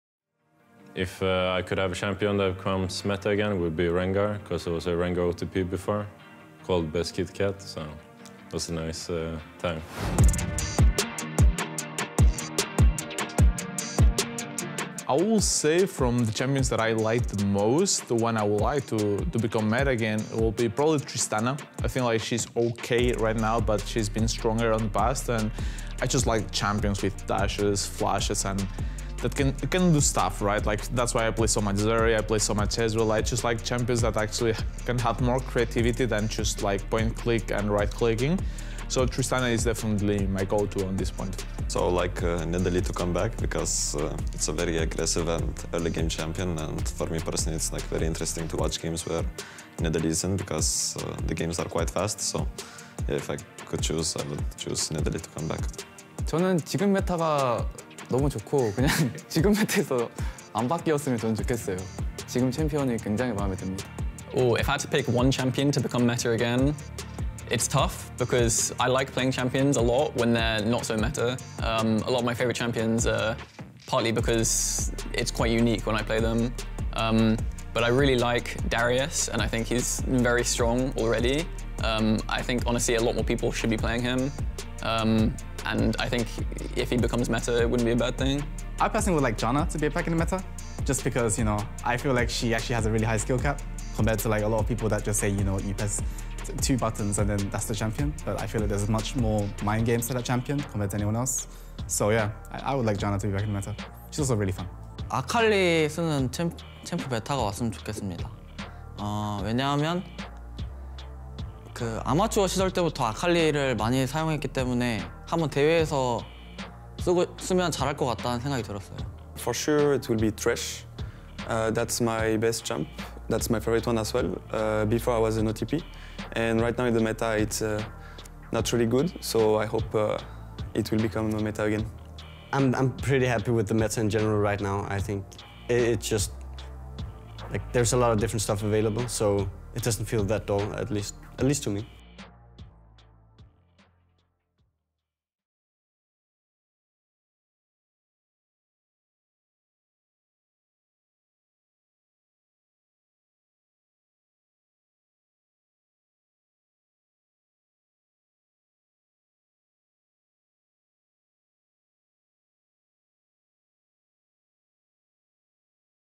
Revive a Champion ft. X7 Esports & Dusty | NLC Finals
Which champion would the X7 and Dusty players like to bring back to the meta? Who would you like to see make a return?